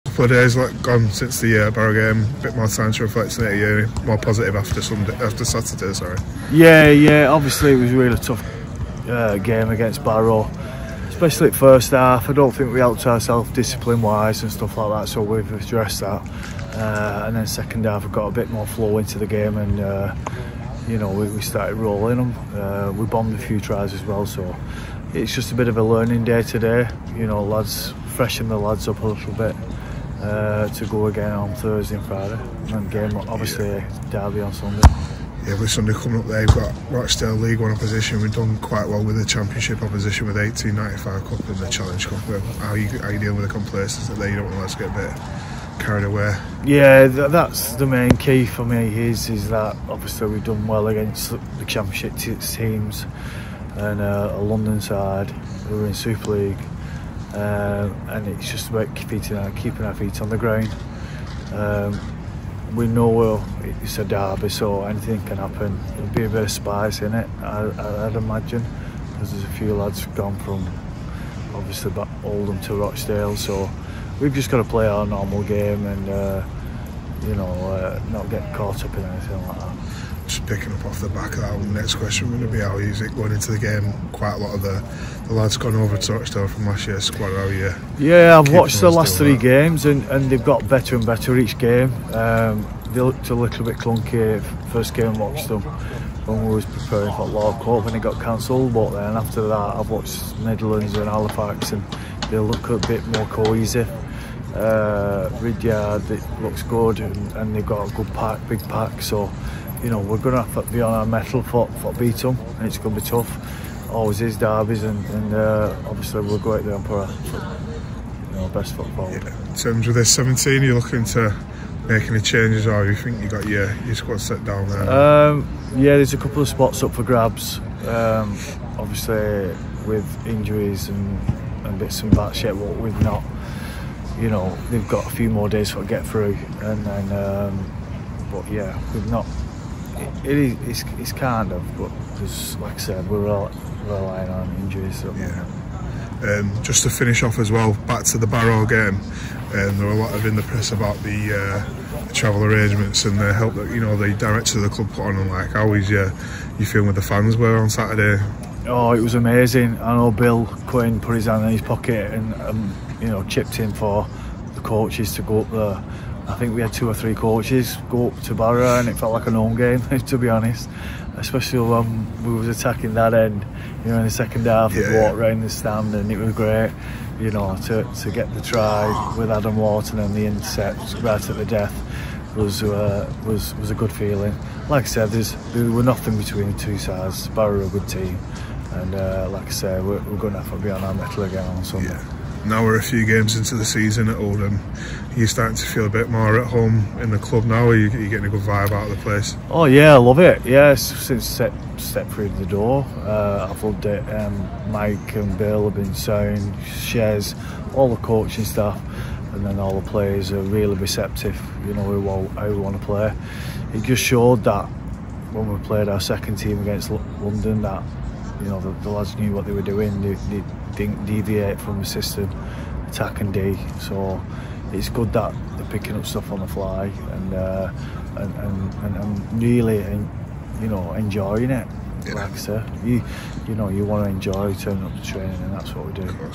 INTERVIEW: Sean Long ahead of Rochdale derby
"We'll have to be on our mettle to beat them."

Sean Long speaks to Roughyeds TV ahead of the derby against Rochdale at Boundary Park this Sunday.